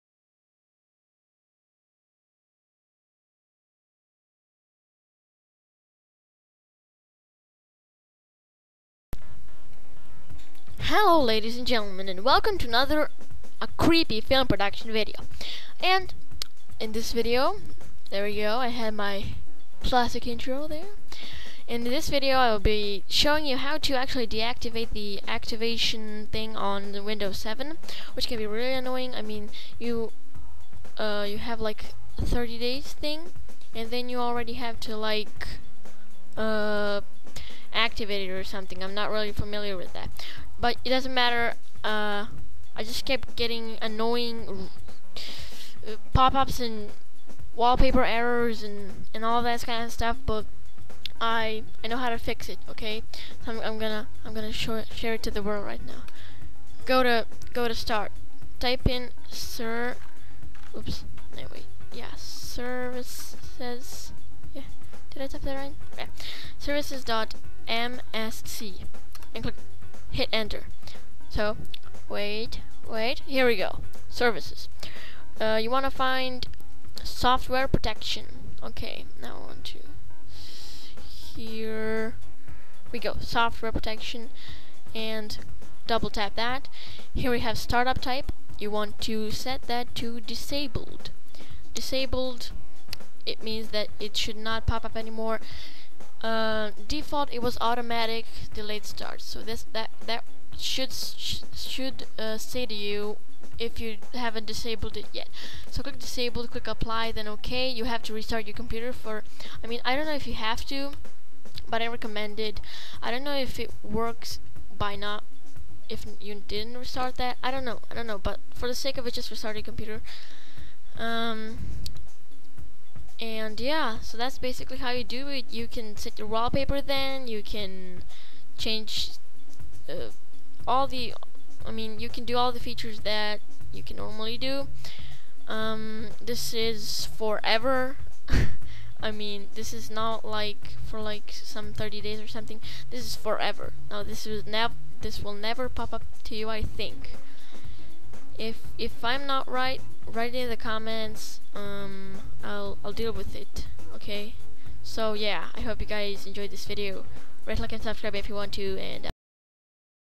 How To Disable Windows 7 Activation
Hi this is another video by Luka Aleksic. Today i'm doing a video of How To Disable Windows 7 Activation a like would be very much appreciated  :)